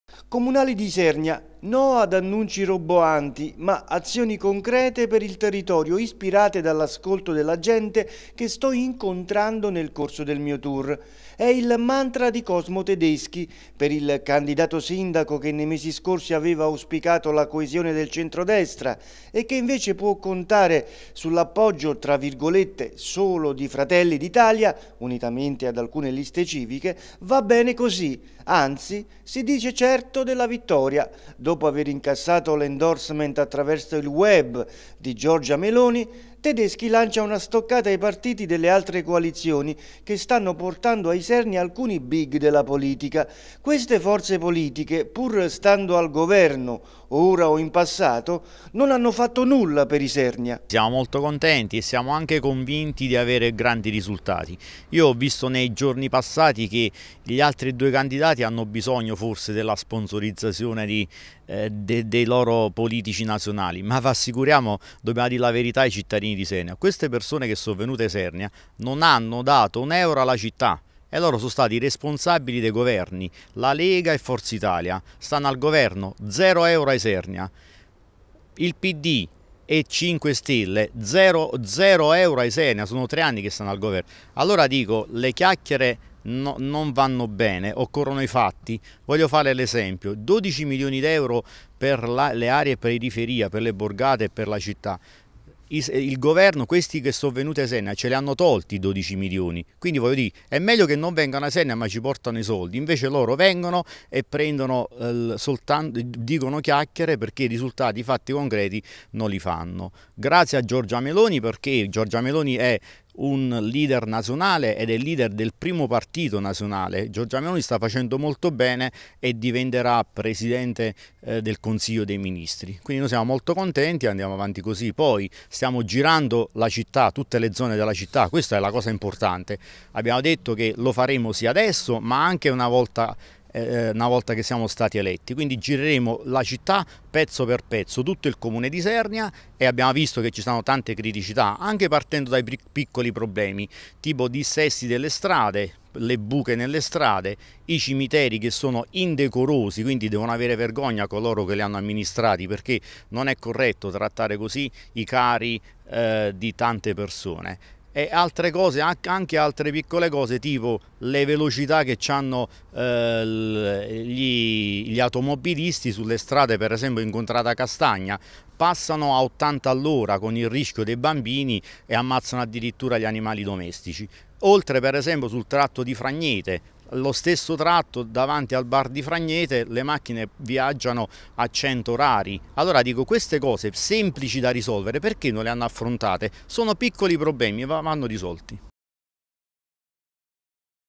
Comunali a Isernia, Cosmo Tedeschi punta sulla concretezza...
Cosmo Tedeschi incassa l’appoggio della Meloni e promette di metter mano ai problemi della città iniziando da quelli  ordinari: “No ad annunci roboanti ma piccole azioni concrete ” le parole del candidato sindaco sostenuto da Fratelli d’Italia